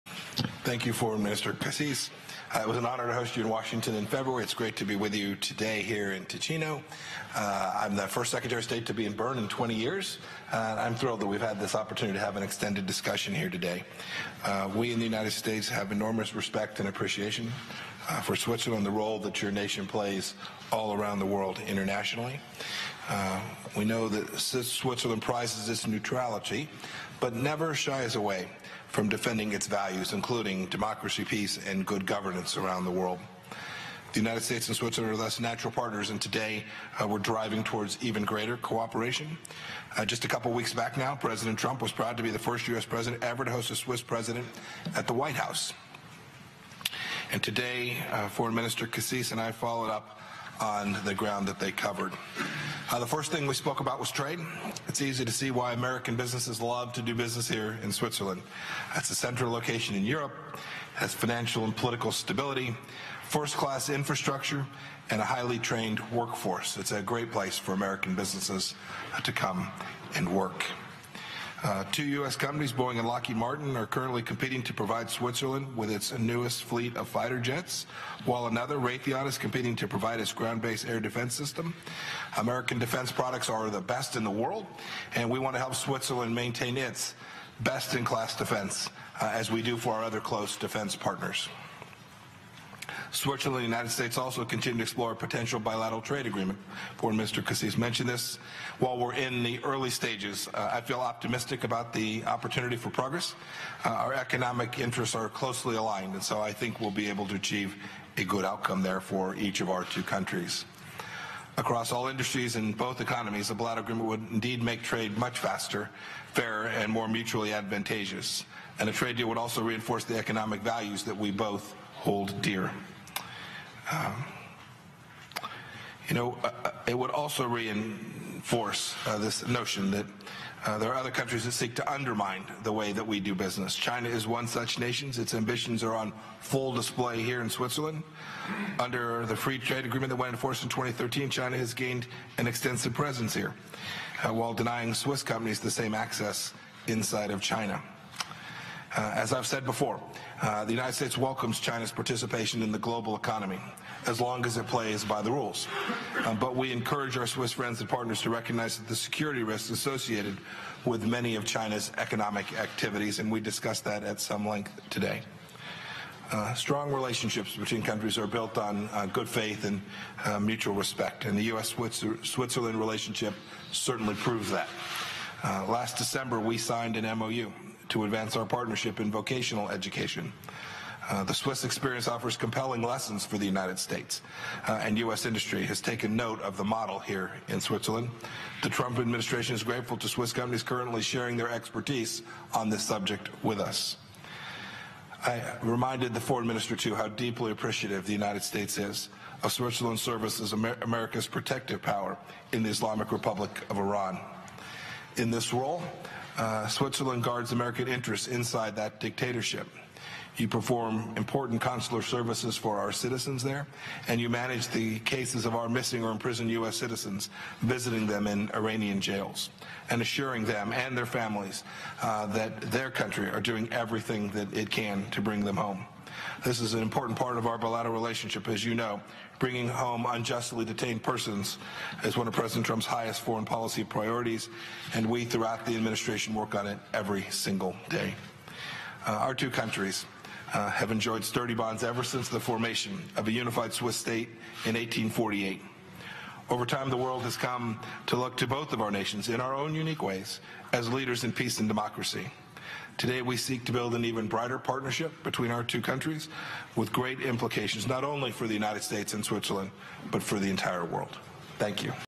Secy Pompeo joint press availability with Swiss FM Cassis
June 2, 2019:  Secretary Pompeo joint press availability with Swiss Foreign Minister Ignazio Cassis, at Castle Grande, in Bellinzona, Switzerland.